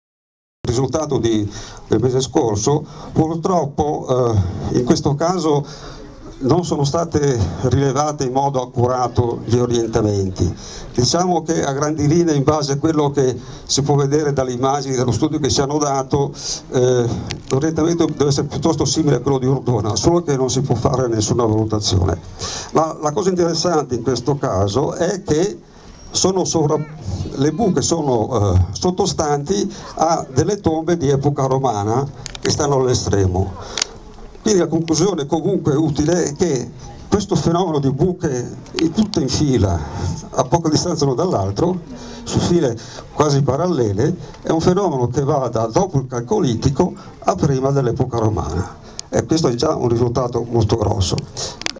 Le tombe di epoca romana di Deliceto ed Ascoli Satriano (Fg)
Al Festival dell'ArcheoAstronomia - Valle dei Templi di Agrigento (tempio di Giunone Lacinia) il professor Elio Antonello con una relazione sulle tombe daunie.